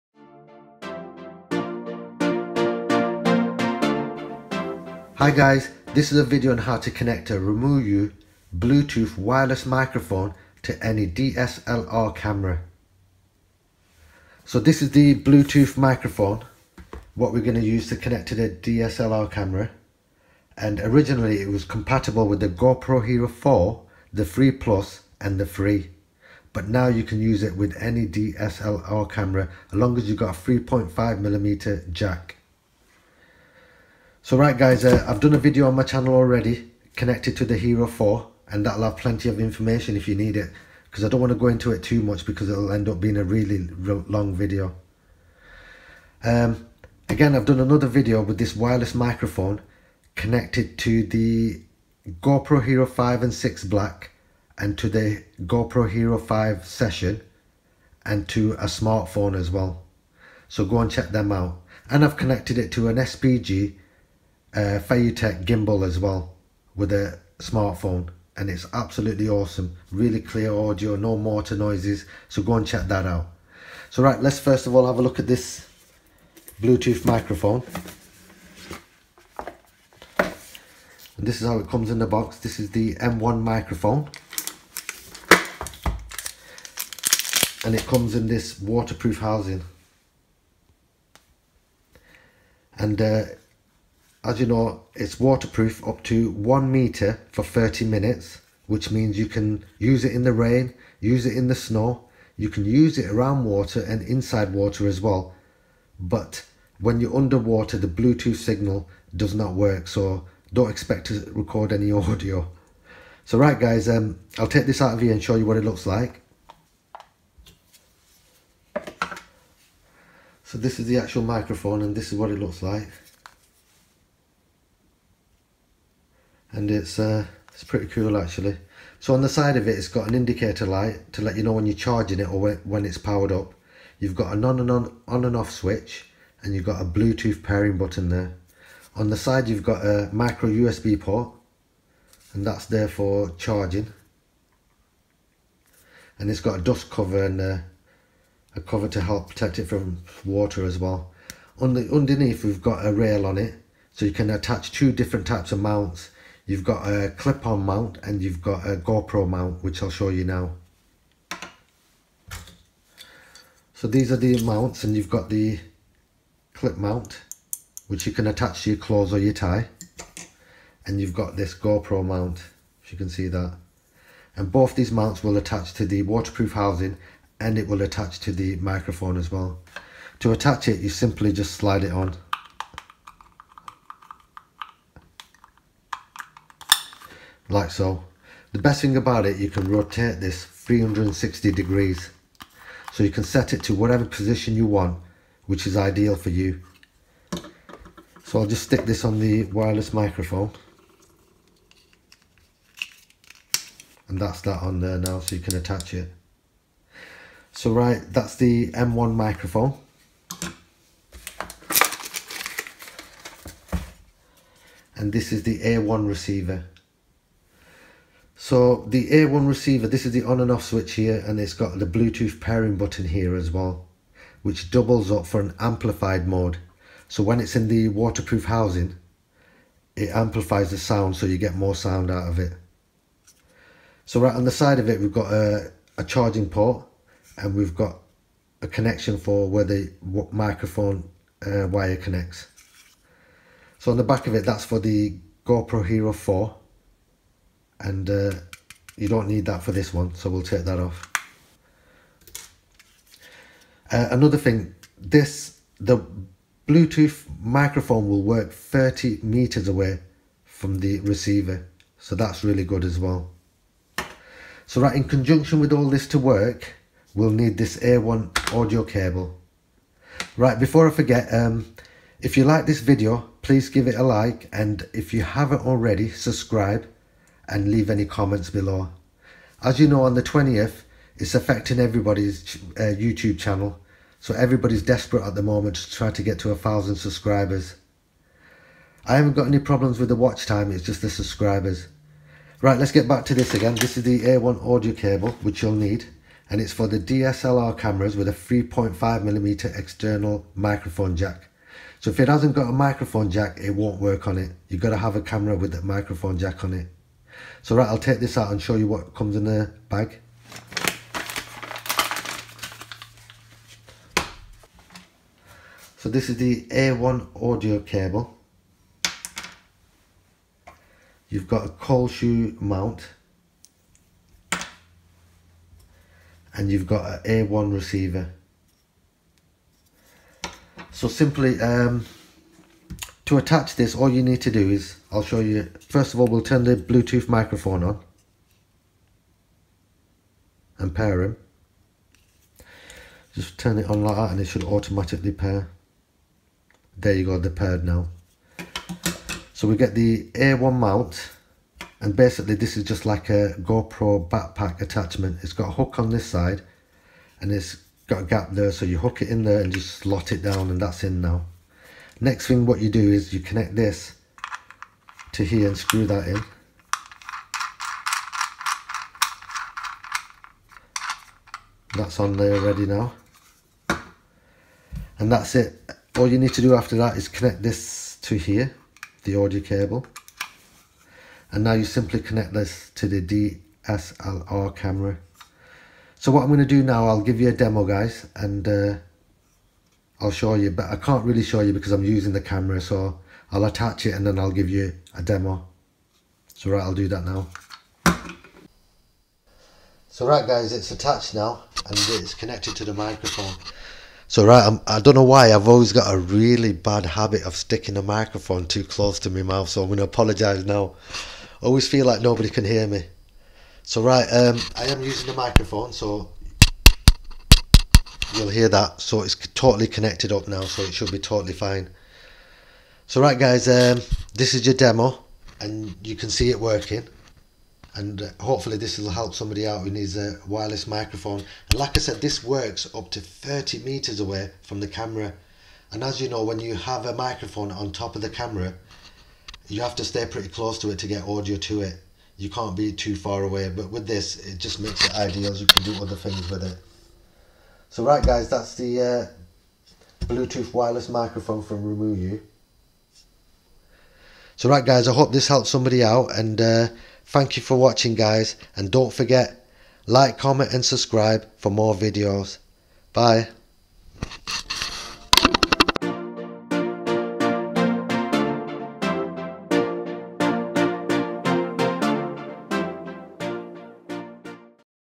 DSLR Cameras Wireless Microphone Removu M1 & A1
Removu Wireless Microphone For DSLR Cameras

Products Used Amazon Links

Click Links For More Information & Or To Purchase

Removu A1 & M1
Removu A1 Audio Cable
Mini Tripod

If You Want A Wired Microphone. Then Try This One Guys. Really Good Quality & Really Good Price Too. Its The Purple Panda Lavalier lapel microphone Kit & You Get Loads Of Thing With It Too. Check It Out Purple Panda Lavalier Lapel Microphone Kit

Hope This Helps Somebody.

Any questions or comments please leave below.
Hope you enjoyed Watching Thank You.